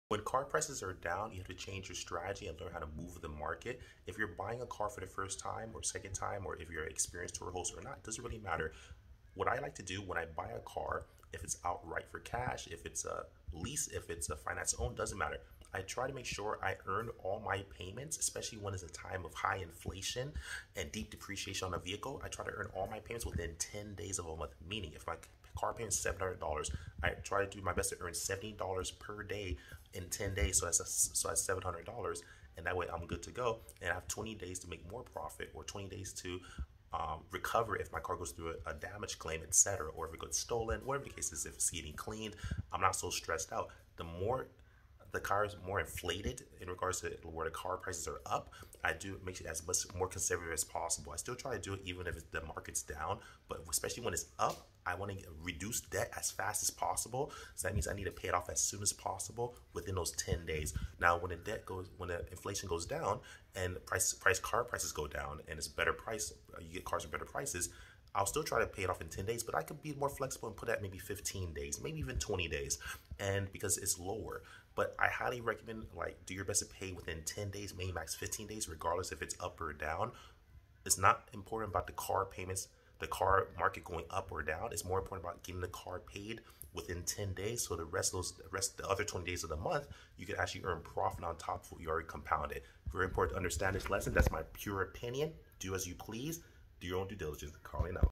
Turo Car Prices Are Down! How To Make Money Now? 🤔
How to take advantage when car prices are down &  build 6 figure Turo business 💰💸

➡️ 💎 LAYN TURO UNIVERSITY GROUP 👨🏿‍🏫🎓 (TURO HOST EXCLUSIVE FACEBOOK GROUP) JOIN